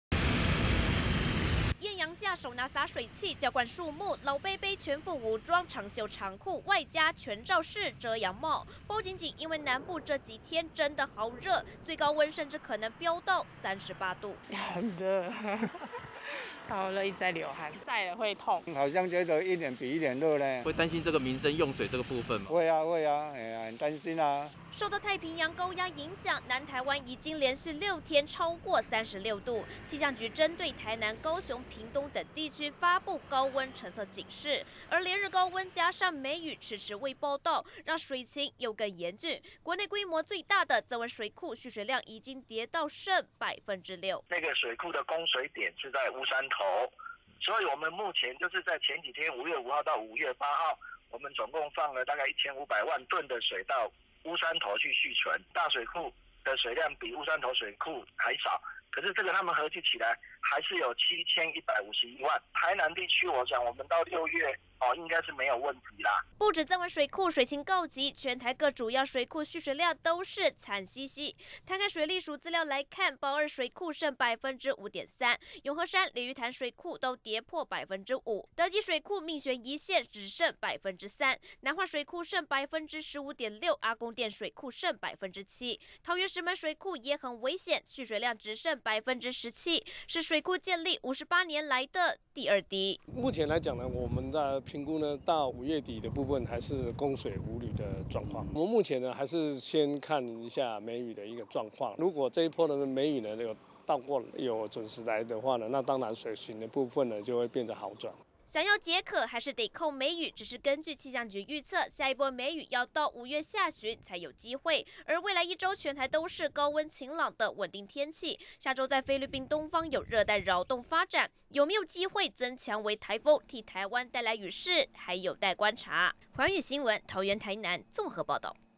水情慘! 曾文水庫剩6% 石門水庫建壩以來第二低｜寰宇新聞 20210510
台灣[水情]持續吃緊，桃園[石門水庫]蓄水量，跌到2成以下，是建壩58年來[第二低]水位，全台最大的台南[曾文水庫]，蓄水量更只剩下百分之6，但根據氣象局觀測，未來一週全台都會是[晴朗炎熱]天氣，也針對[台南、高雄、屏東]等地區發佈高溫警示，要等到5月下旬，才有機會盼到[梅雨解渴]。